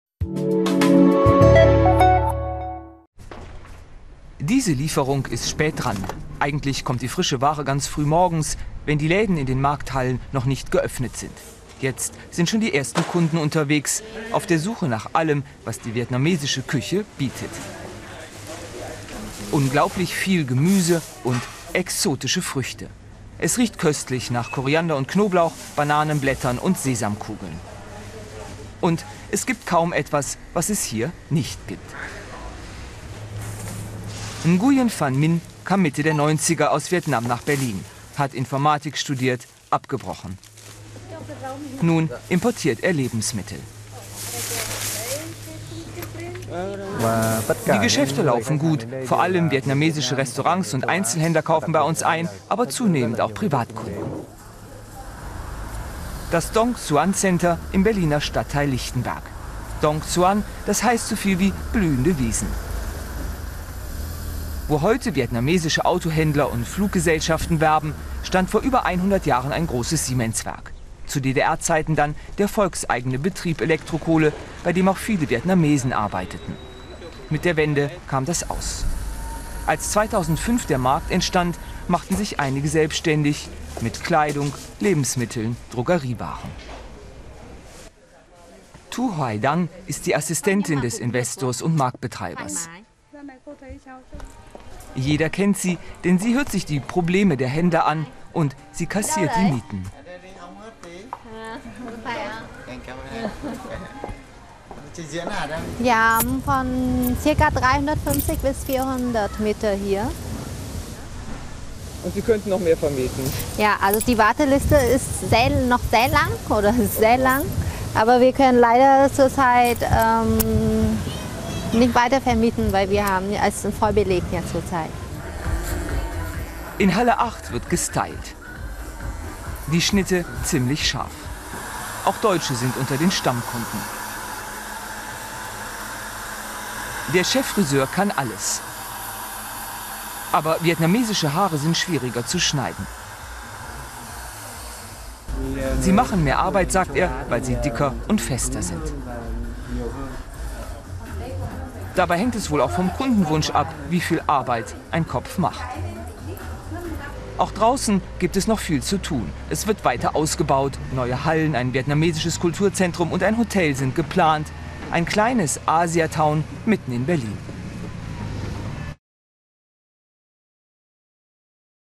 Klein-Hanoi in Berlin | Wirtschaft kompakt
Berlin ist nicht nur die größte türkische Stadt im Ausland, sie ist auch die größte vietnamesische Stadt in Deutschland. Das macht sich Nguyen Van Minh zu Nutze: Er baut Hallen, in der hunderte Händler vom Dessous-Laden bis zum Feinkostgeschäft, von der Technik bis zum Fischhändler an ihn Miete zahlen.